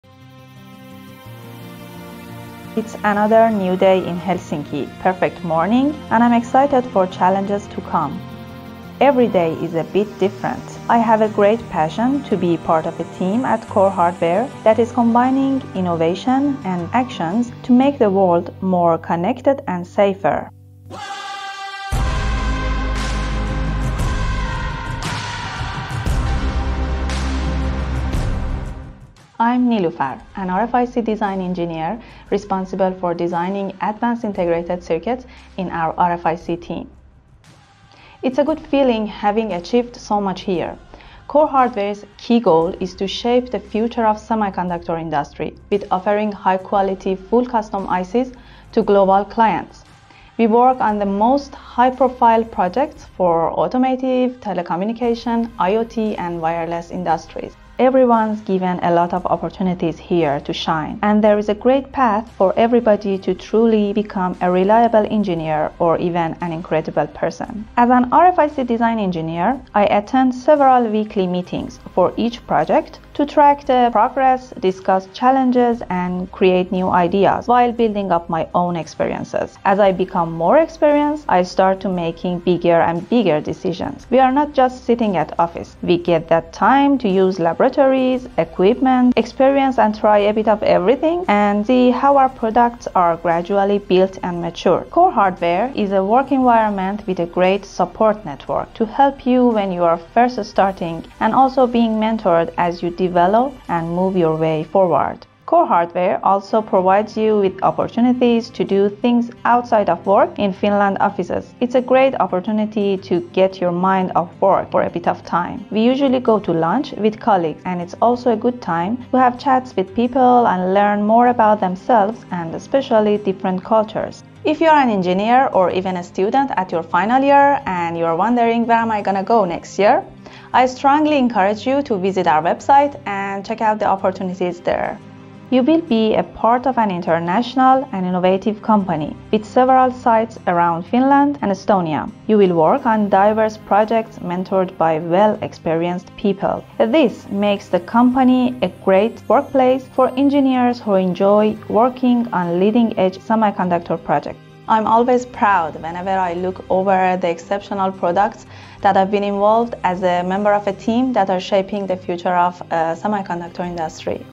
A Day in the Life of a Design Engineer at CoreHW - Niloofar Sharifi
This video will help you to understand what is a day in the life of being an RFIC design engineer. Niloofar will discuss the importance of daily meetings, outside the work activities, communication, and what needs to be done everyday at CoreHW.